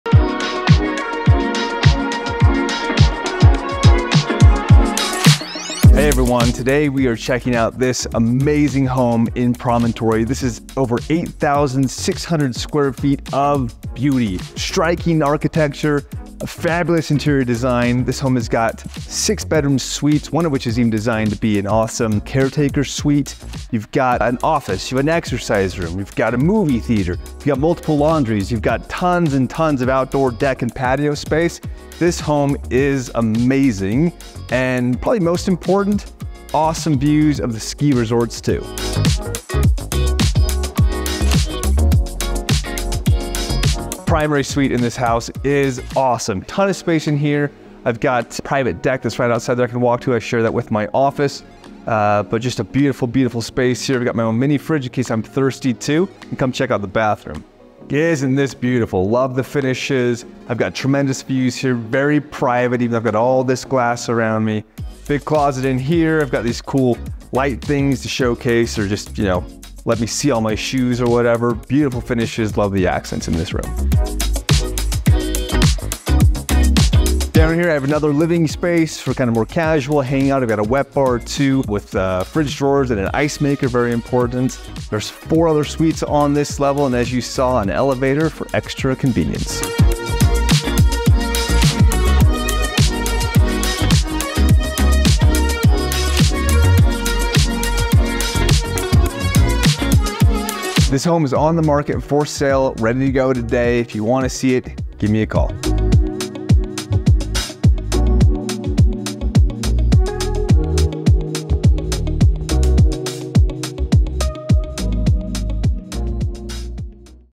2687 Canyon Gate Rd | Promontory Park City
This impressive estate home in Park City offers an unparalleled luxury experience. Crafted by the renowned custom home designer James L. Carroll, this luxurious abode was meticulously designed to maximize the breathtaking views of the majestic Park City and Deer Valley mountains from every window in the house. Boasting over 8,600 square feet of opulent living space, this stately property sits atop Promontory Ridge in Promontory Club and offers stunning panoramic vistas all around. With a grand glass elevator accessing each of the three levels with ease, this architectural masterpiece exudes an air of opulence and sophistication. An amazing sanctuary for those seeking a one-of-a-kind mountain retreat, it's astounding beauty will leave you awe-inspired and looking to become a local. From the soaring 16-foot ceilings to the 30-foot automatic sliding glass doors, every inch of this property evokes an impressive and inviting atmosphere. Six en-suite bedrooms, nine bathrooms, a study, a fitness room and private movie screening theatre are all perfectly complemented by four fireplaces and two outdoor firepits; making this among the finest properties in Park City. Take advantage of over 3,100 square feet of outdoor living spaces, with ample amenities for entertaining guests or simply admiring the sunsets from multiple decks and balconies. And for the car enthusiast, you'll love the 5-plus car garage finished with room for a golf simulator or car turntable. Interiors and finishes are curated by Bond Design Group. Plus, as an added bonus, a full golf membership is available immediately - no waitlist necessary! This is your chance to own a truly remarkable estate home like none other.